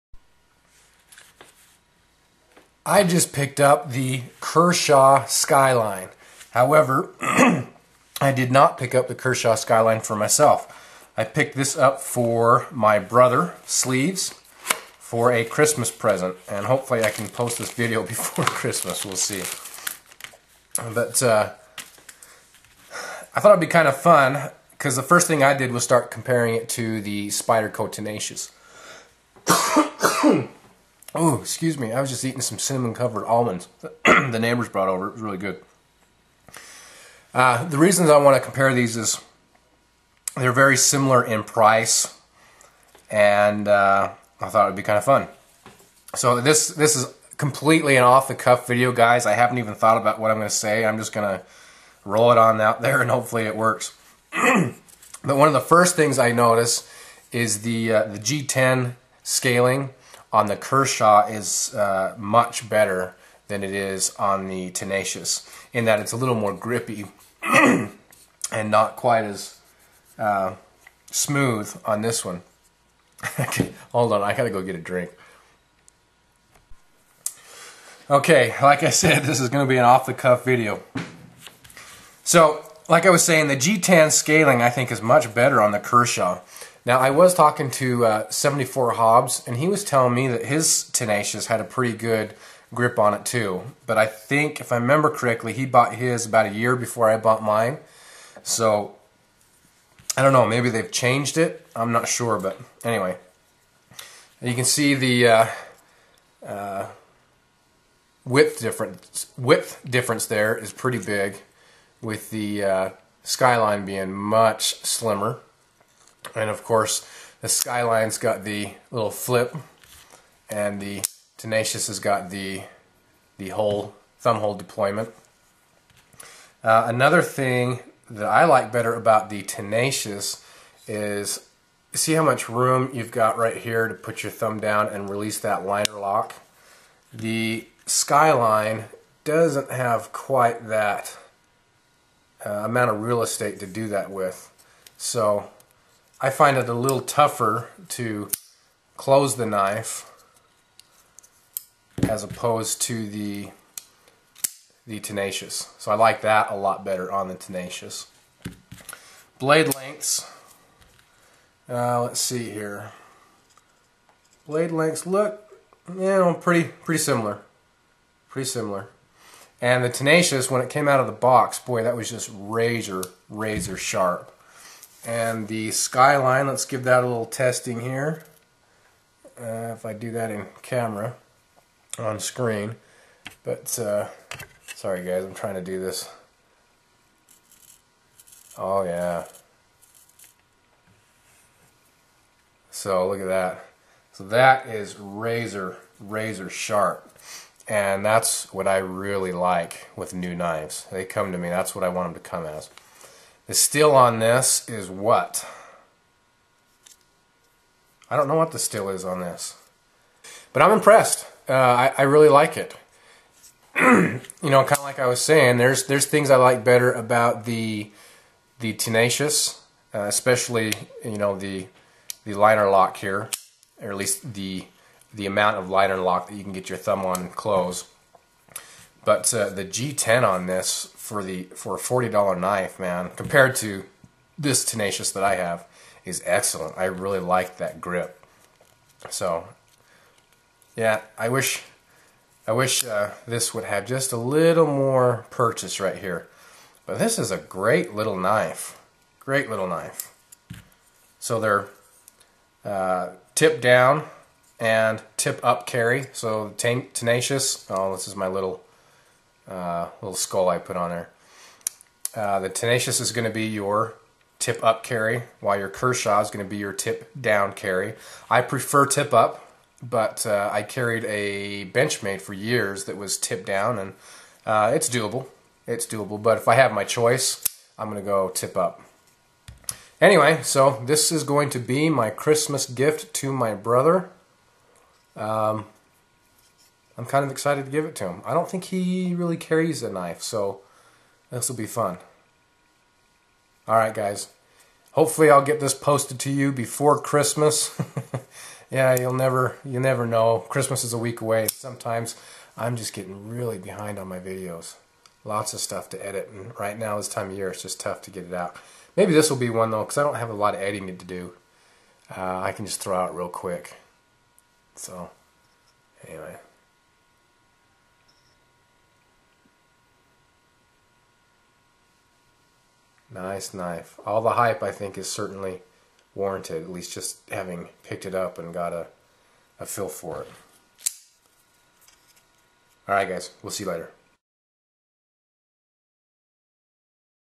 Kershaw Skyline - Initial Thoughts (vs the Tenacious)
I do not consider this a review of the Skyline, just my initial thoughts. While the steel is very sharp (as you'll see in vid) I would like to know how it wears/how easy to sharpen with my meager skills, and how well it holds up to the elements as I have no experience with the steel. I did find it a little cheaper on line than the $35 I paid, but after shipping they would come out very similar (and I would have to wait for delivery. Going to the family Christmas party tonight). Bottom line is this, I want one.